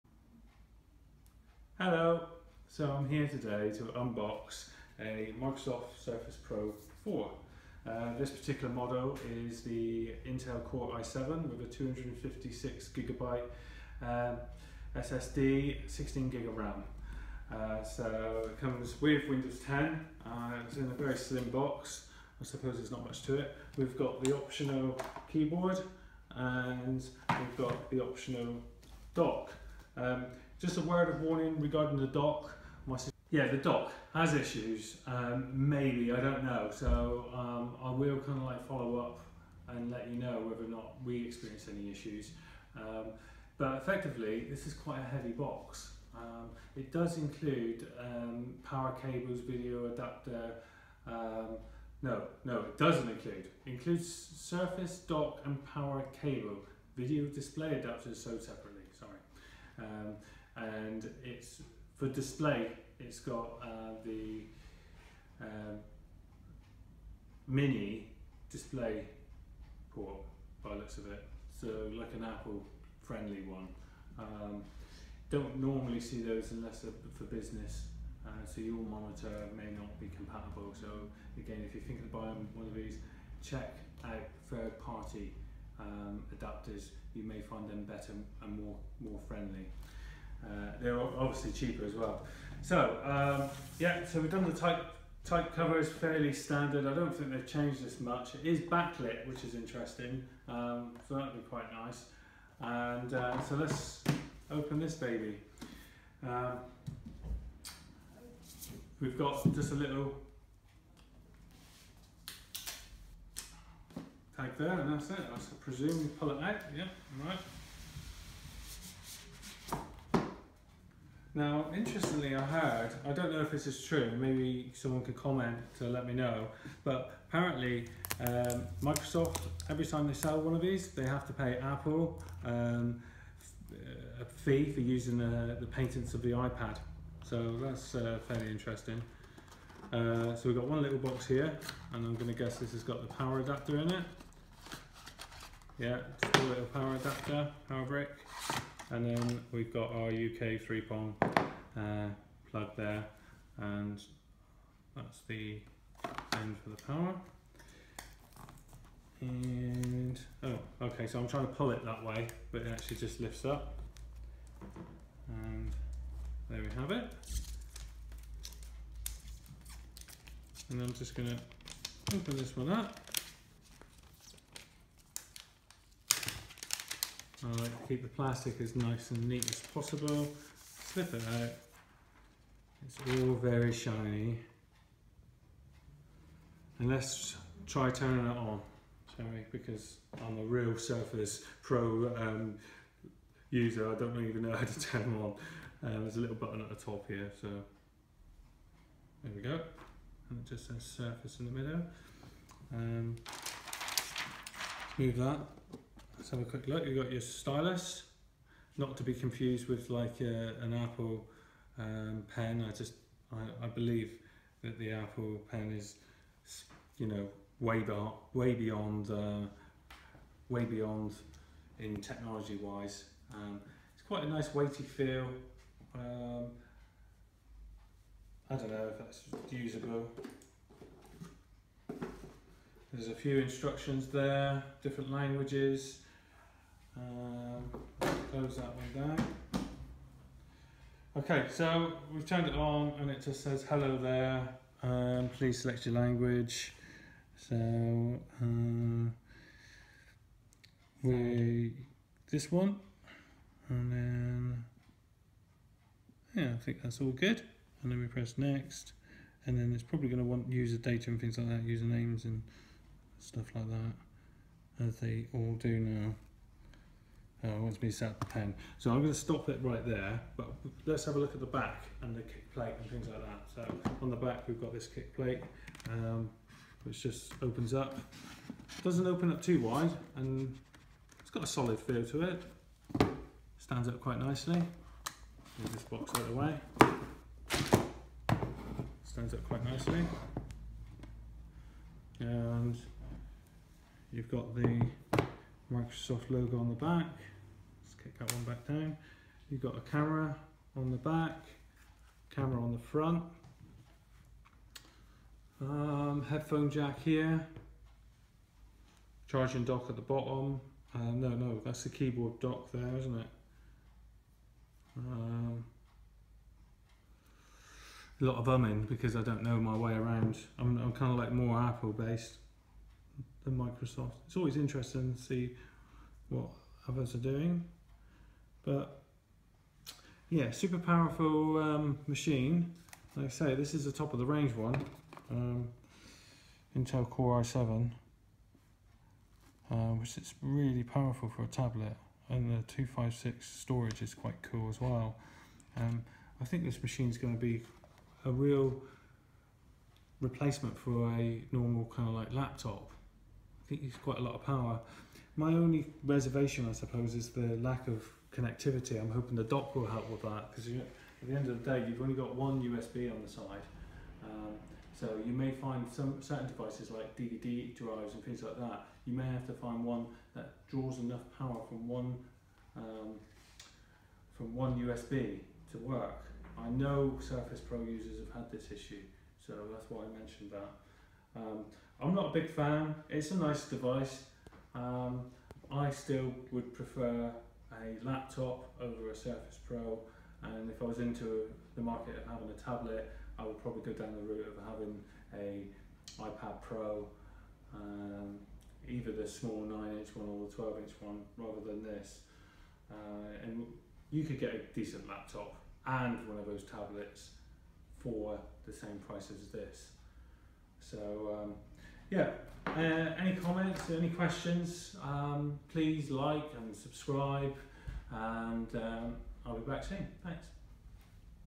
Microsoft Surface Pro 4 Unboxing with Keyboard & Dock by Apple User
Microsoft Surface Pro 4 with Intel i7 256gb SSD & 16gb Ram
Unboxing with keyboard and mouse.